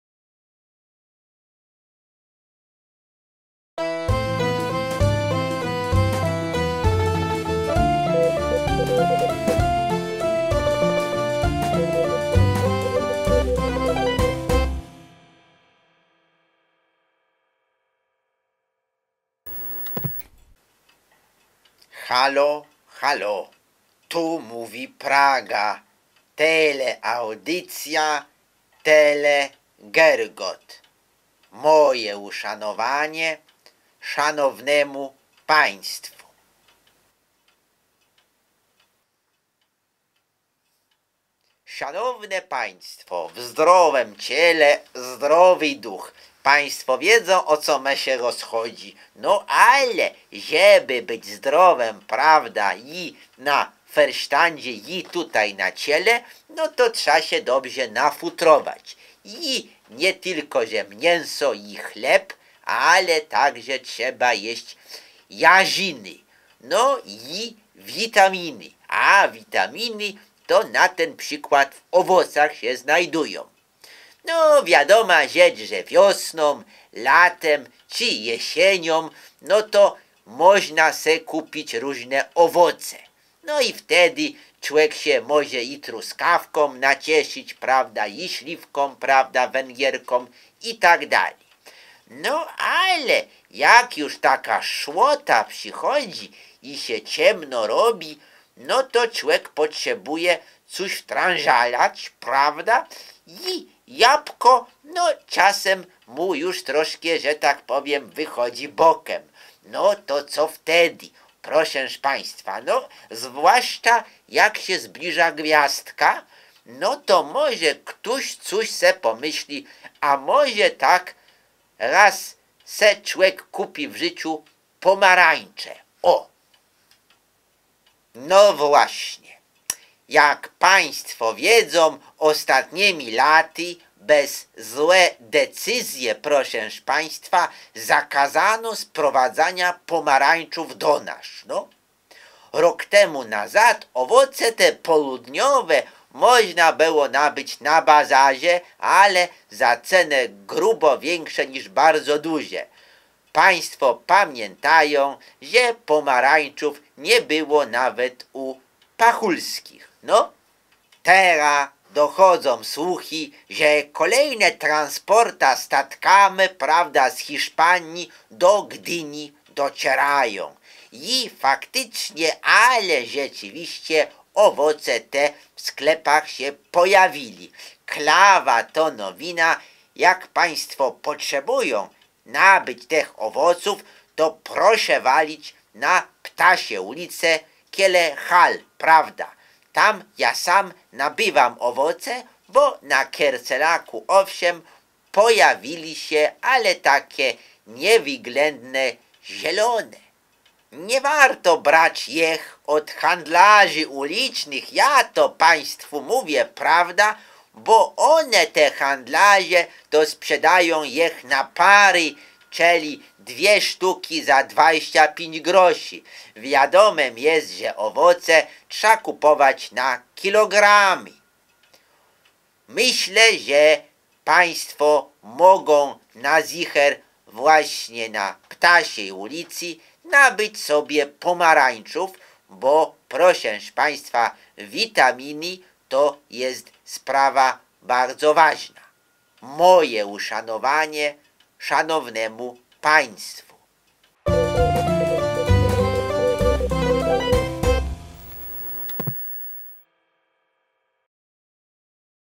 Szmalec i pomarańcze
Telewizja Tele-Giergot emituje na falach ultrakrótkich tele-audycję, w chtórej
tele-speakier Piotr Giergot zakłada mowę o tem, czego brakuje w sprzedaży na bazarze.

Teleaudycja emitowana jezd legularnie z budenku na Pradze

Muzykie do tele-audycji uskutecznił : Piotr ,,Ziarek'' Ziarkiewicz.

Uwaga!

Teleaudycja ma charakter edukacyjno-humorystyczny.

Tele-speakier Piotr Giergot posługuje się kombinowanem językiem polskiem - m.in. tak zwana gwara warszawska uliczna i gramatyka polska sprzed reformy z 1936 roku a często i gięsto jeszcze z wieku XIX.

Mowe takie można było usłyszeć na targowiskach i ulicach Warszawy i Pragi a także przedmieść Sereniego Grodu

@Piotr Giergot Najnowsza teleaudycja Telegiergot